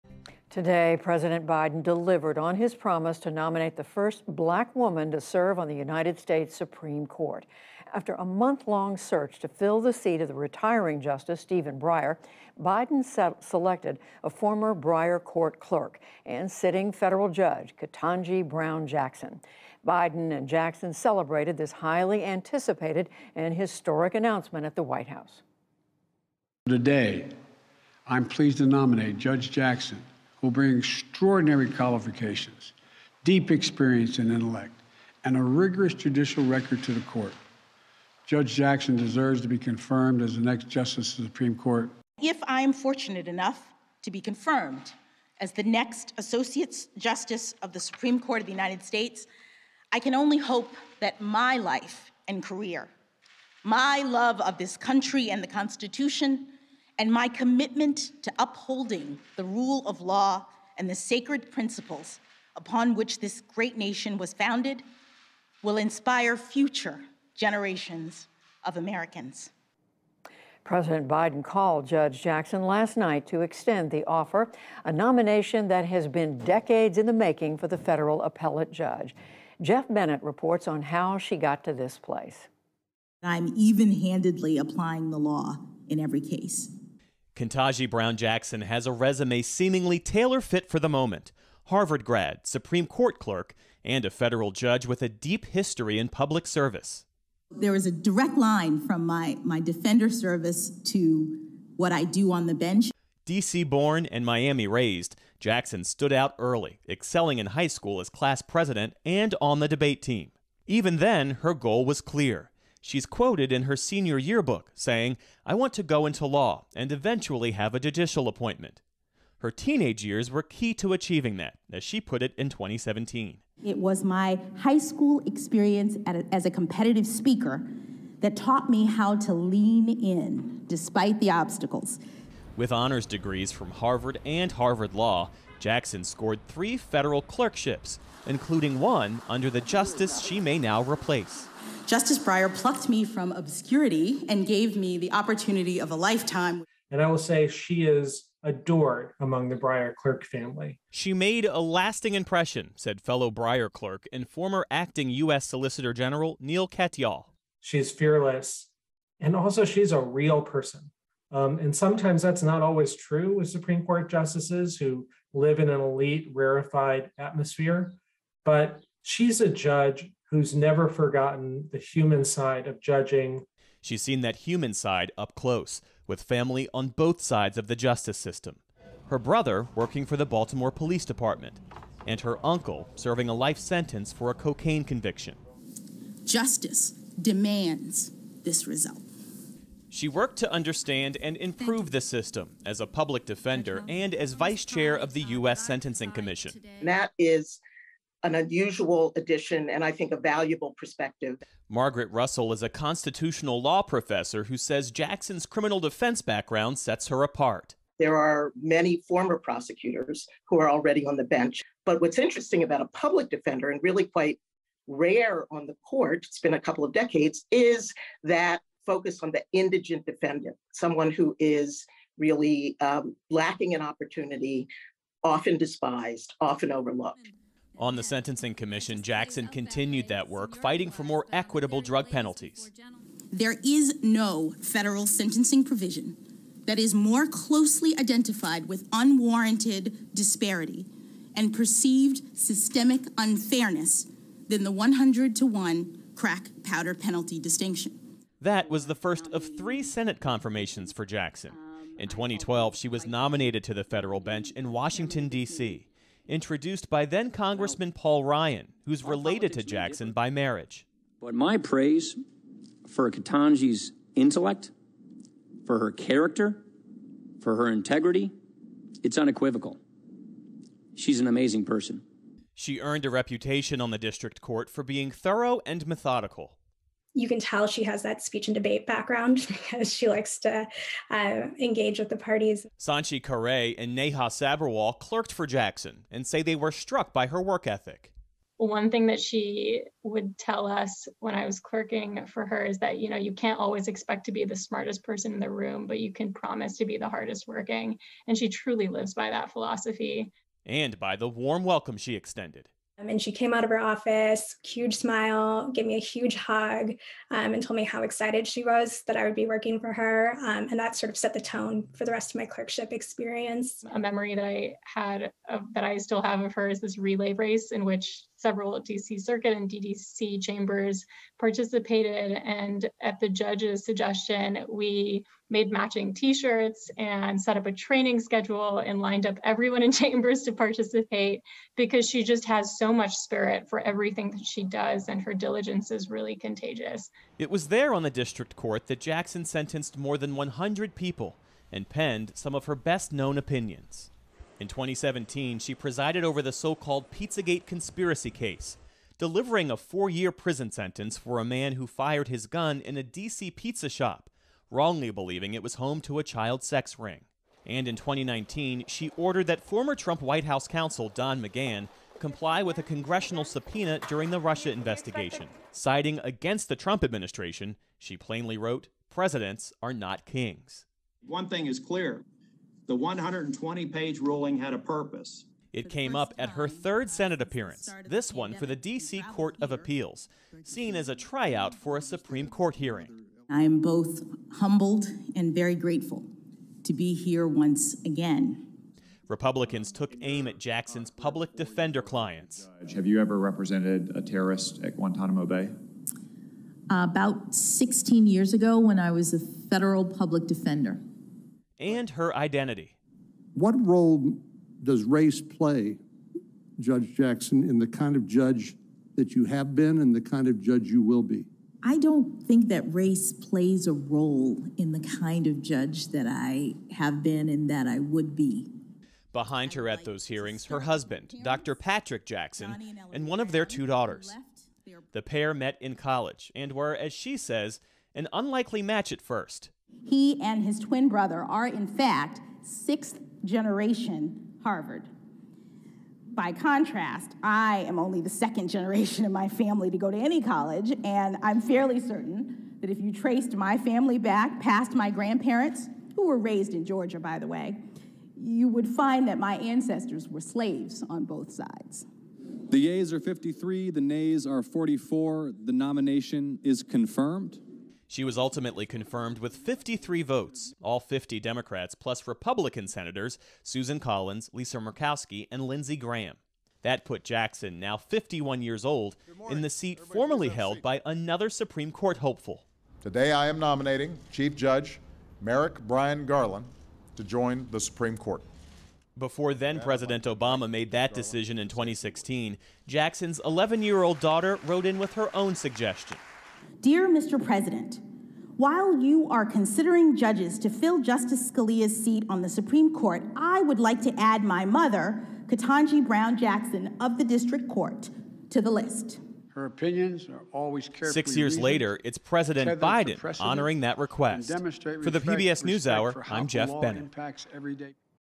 Biden selects Ketanji Brown Jackson for the U.S. Supreme Court
President Biden on Friday delivered on his promise to nominate the first Black woman to serve on the United States Supreme Court. After a month-long search to fill the seat of retiring Justice Stephen Breyer, Biden selected a former Breyer clerk and sitting federal judge, Ketanji Brown Jackson. Geoff Bennett reports on how she was chosen to receive a nomination that was decades in the making.